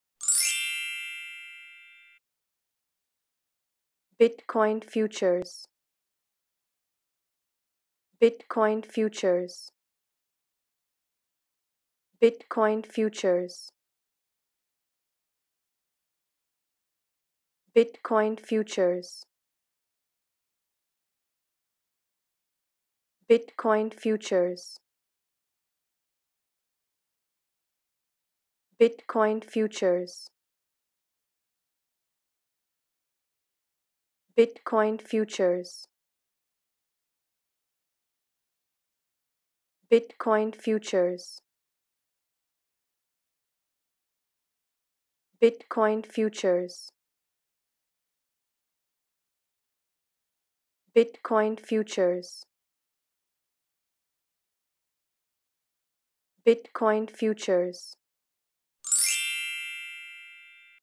How to Pronounce Bitcoin Futures
Learn how to pronounce the word 'Bitcoin Futures' with Unstammer's free pronunciation tutorials. Also read the meaning(s) and usage of Bitcoin Futures